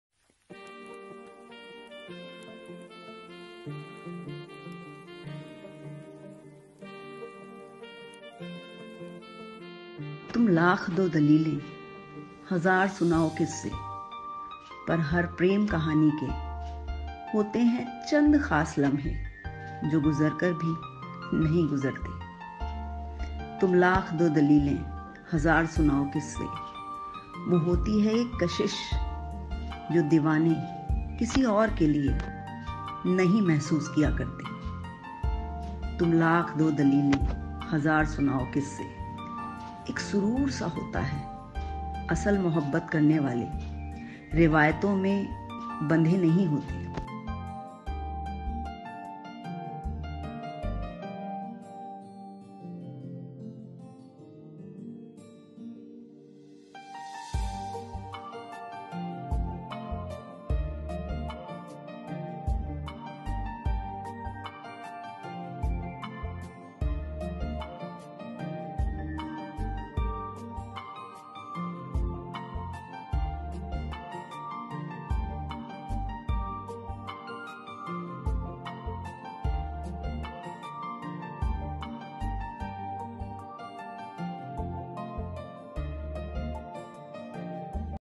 लाख दो दलीलें …
A Hindi poem by my pen and recitation by myself .